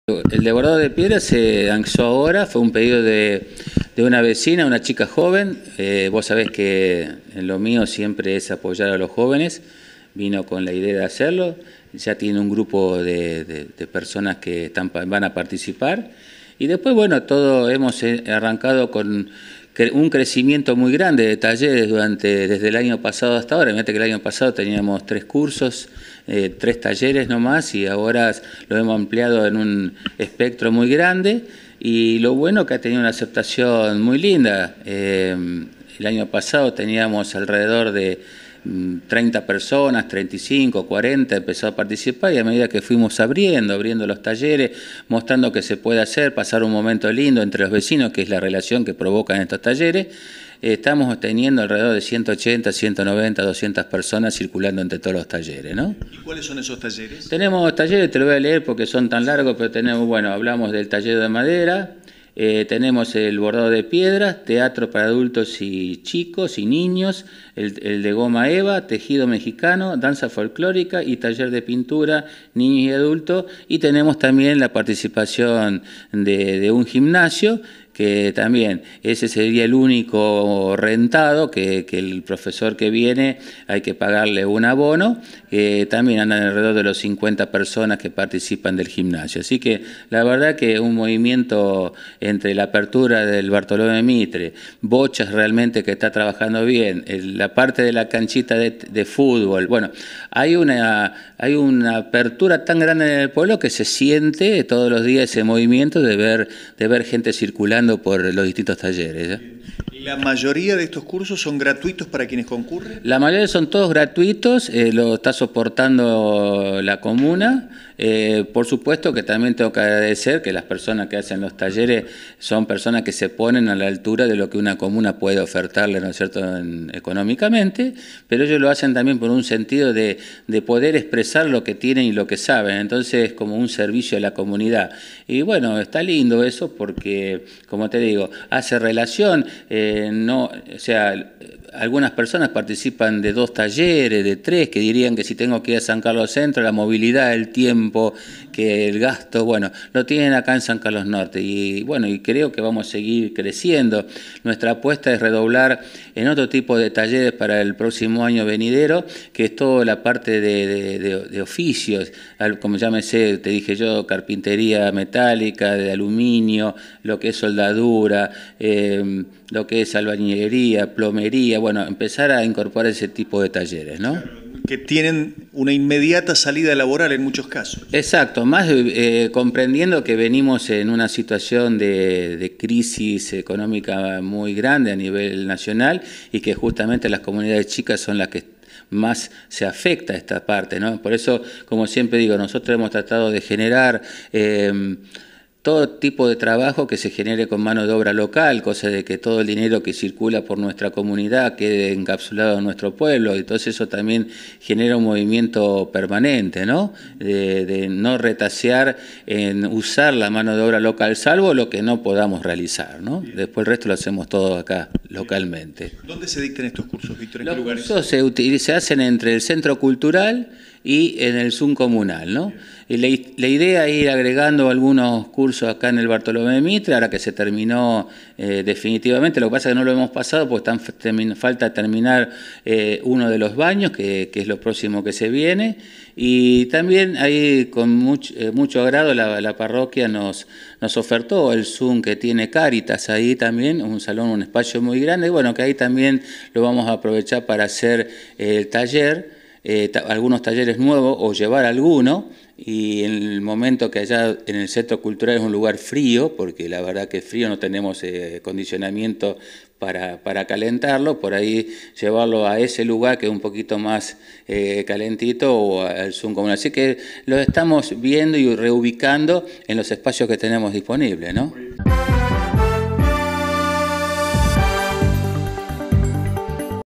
VÍCTOR CAVALLERO – PTE. COMUNAL
TALLERES COMUNALES QUE SE BRINDAN EN LA LOCALIDAD